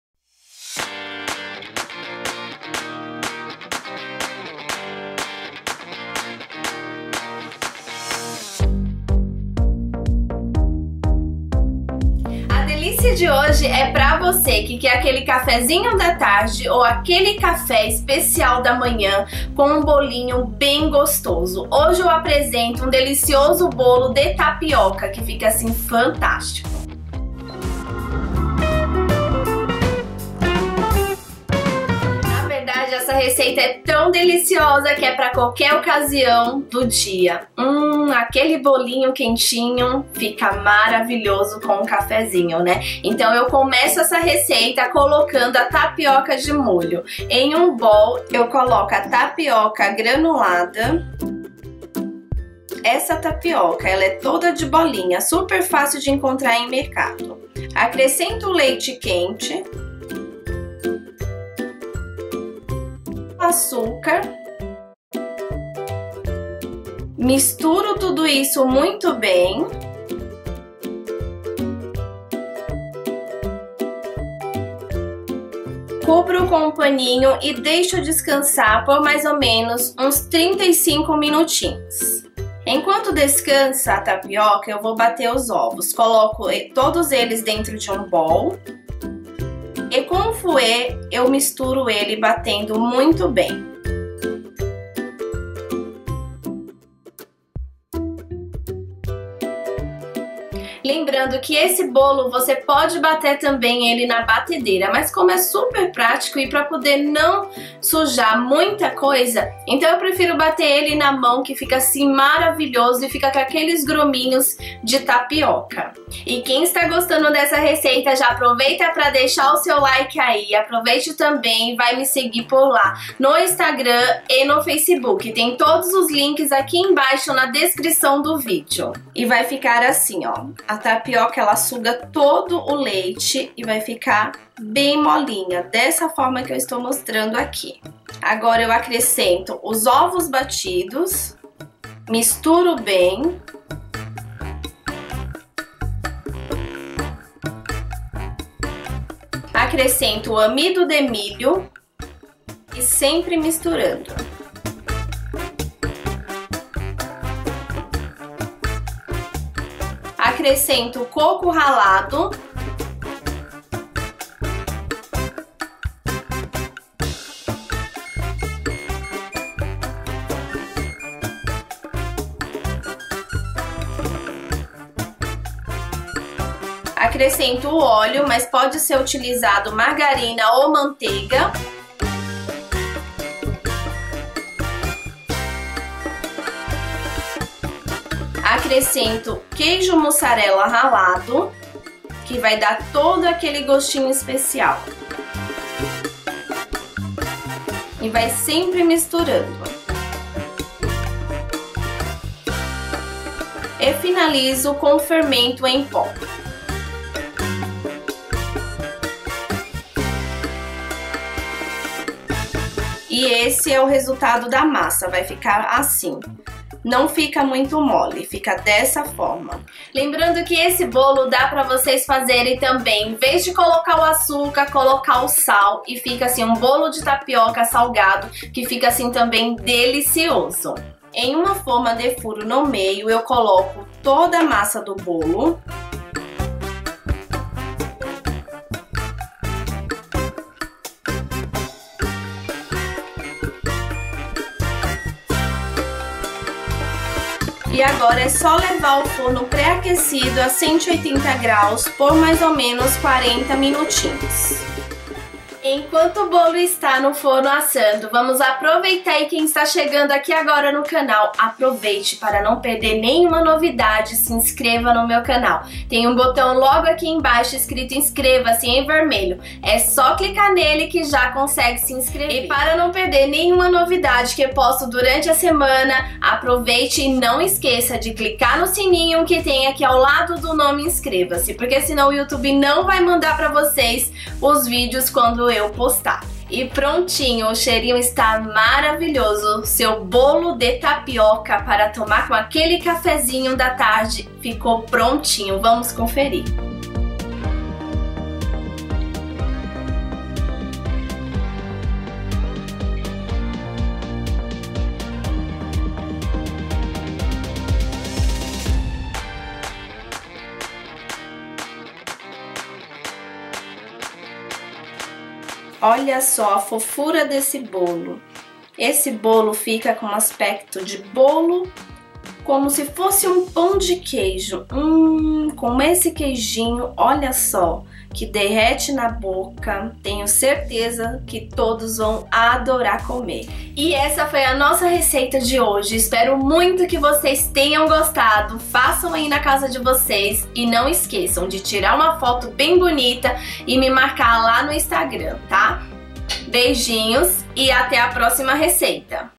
BOLO DE TAPIOCA FÁCIL
Para enviar mimos:
Endereço: Estrada Do Copiuva 1500 Apto 17 torre Lua
Bairro: Vila da Oportunidade   Carapicuiba- SP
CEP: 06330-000

📝INGREDIENTES

500gr de tapioca granulada
800 ml de leite quente (bem quente)
1 xícara (chá) de açúcar
3 ovos
1 colher (sopa) de óleo ou 2 colheres (sopa) de margarina
5 colheres (sopa) de amido de milho
1/2 xícara (chá) de coco ralado
200gr de queijo mussarela ralado
1 colher (sopa) de fermento em pó

Medida da minha xícara 240ml.